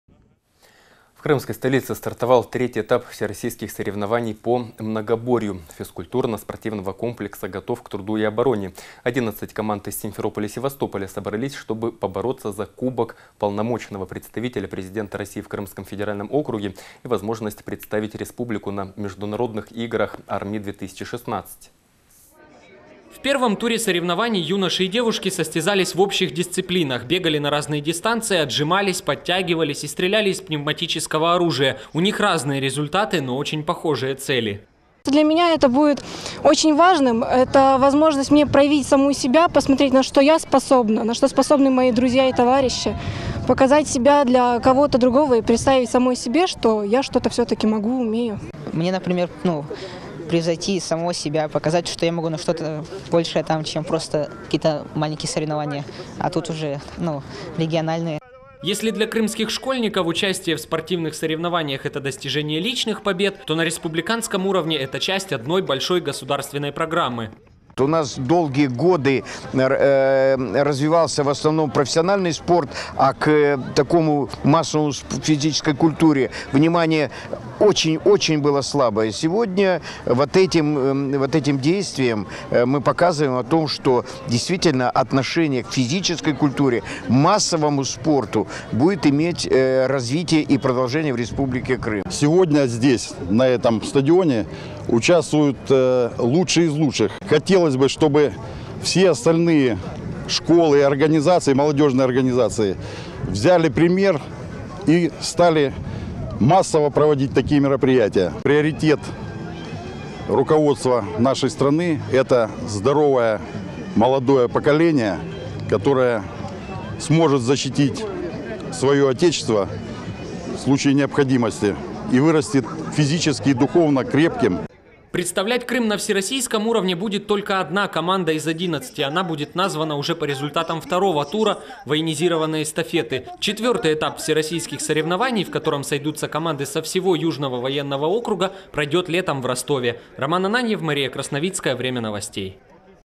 На пути к победе
В крымской столице стартовал III этап всероссийских соревнований по многоборью физкультурно-спортивного комплекса «Готов к труду и обороне». 11 команд из Симферополя и Севастополя собрались, чтобы побороться за кубок полномочного представителя Президента Российской Федерации в Крымском федеральном округе и возможность представить республику на международных играх «АрМИ-2016».